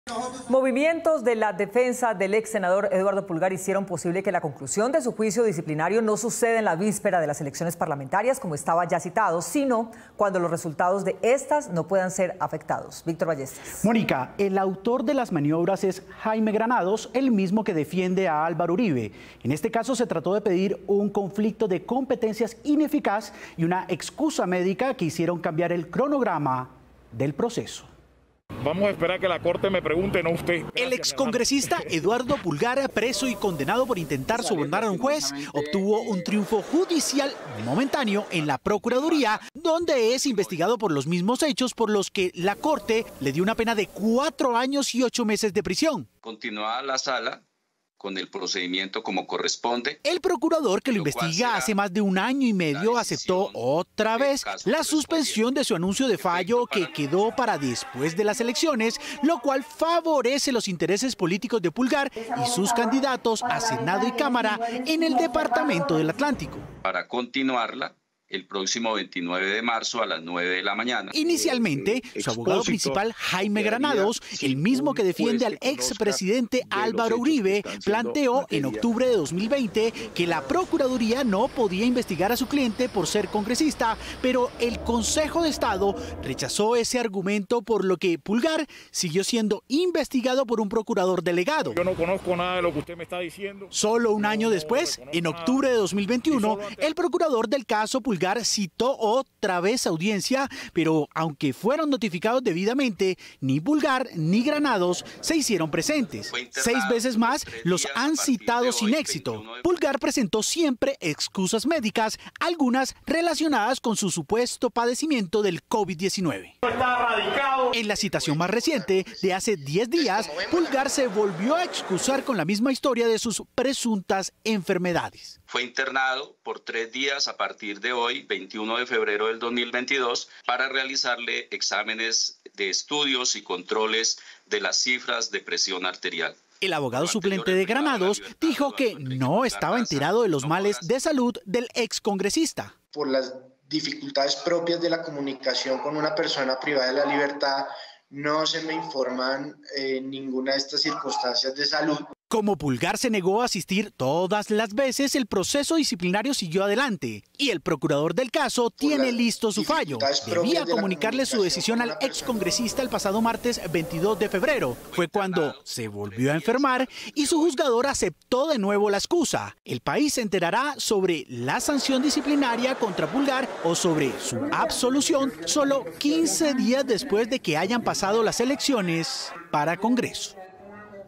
Caso Pulgar en Procuraduría: seis audiencias y todas se aplazaron.
Movimientos de la defensa del exsenador Eduardo Pulgar hicieron posible que la conclusión de su juicio disciplinario no suceda en la víspera de las elecciones parlamentarias, como estaba ya citado, sino cuando los resultados de éstas no puedan ser afectados.